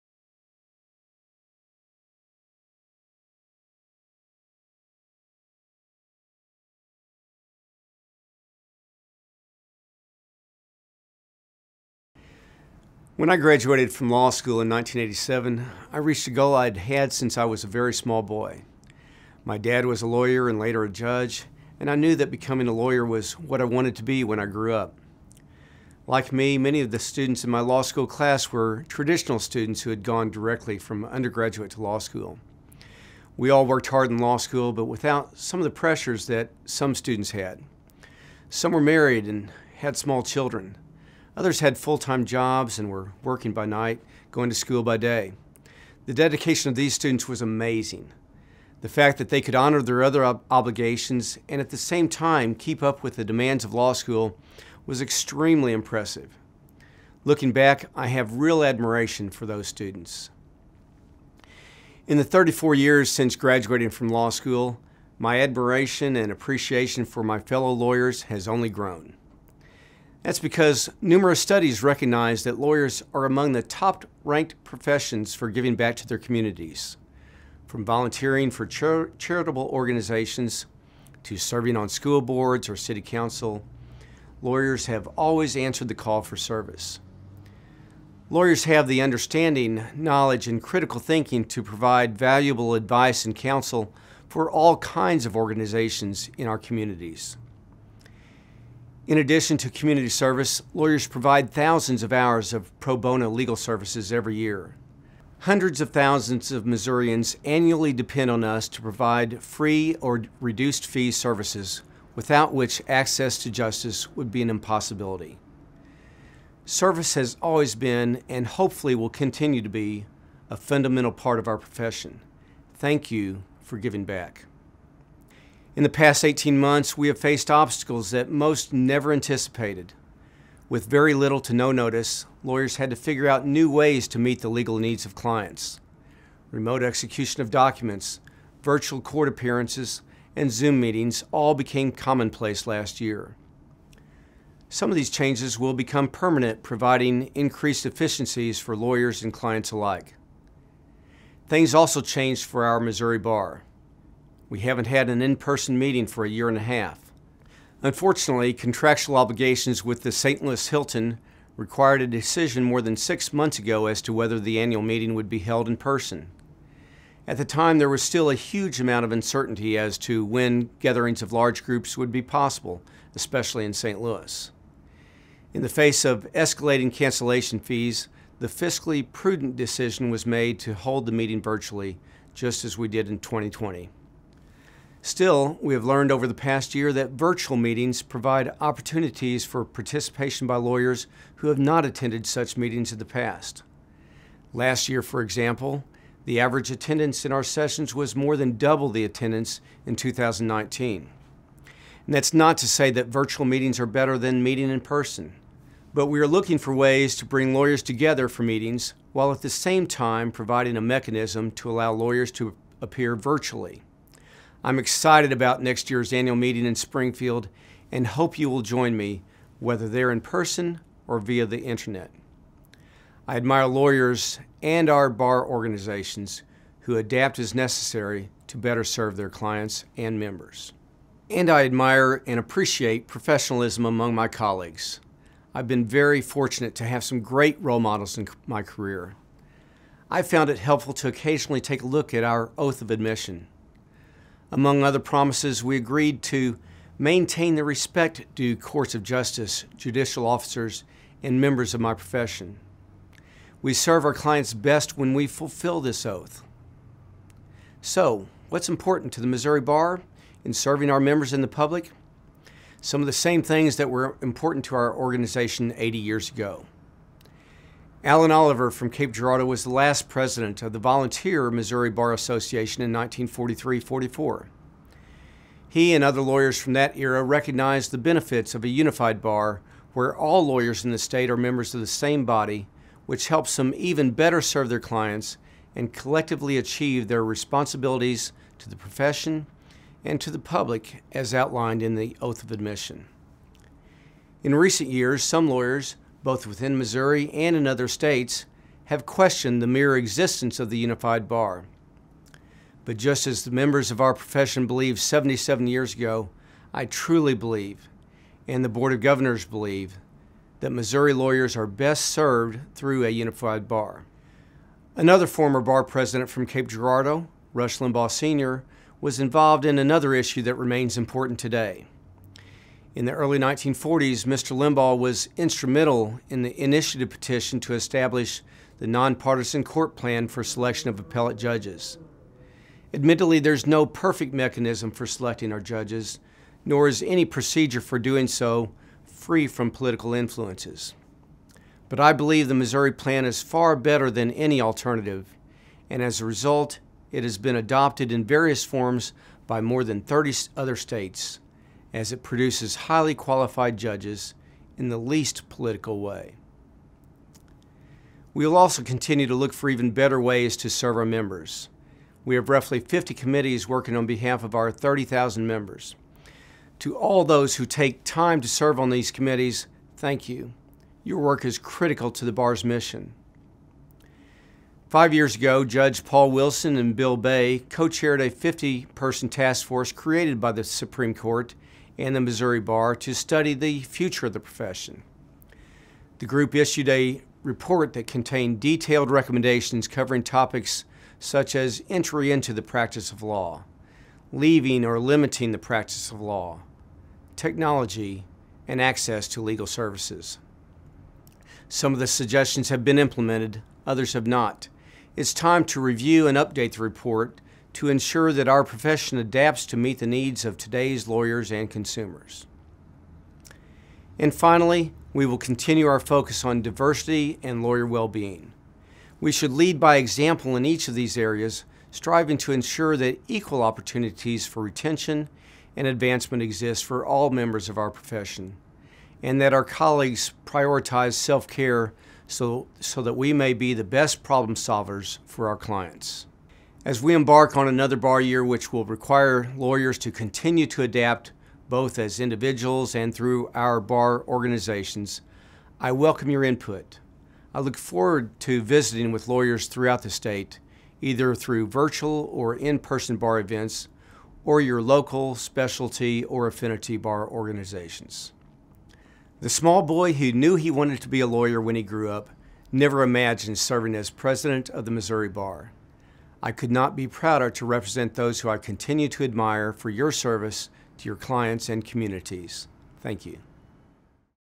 2021 Virtual Annual Meeting Closing Video
Remarks from John Grimm, Missouri Bar President 2021-22 .